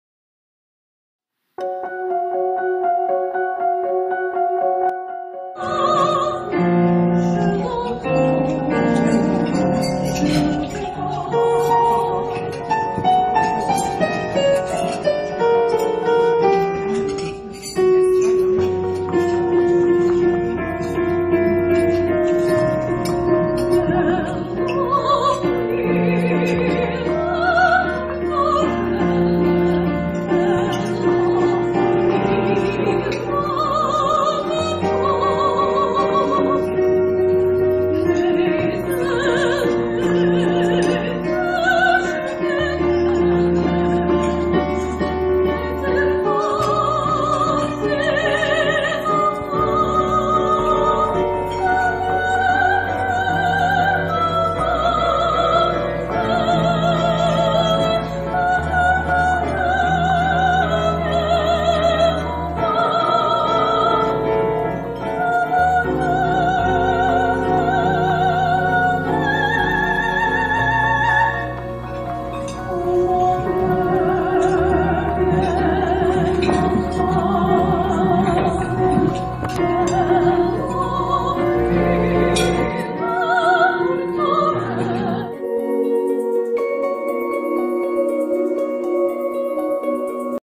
Kristelyn Blough - Opera Singer in Charleston, SC
Kristelyn Blough performs Opera Program at Greater Charleston Rotary Auxiliary Club.  Charleston, SC April 2015.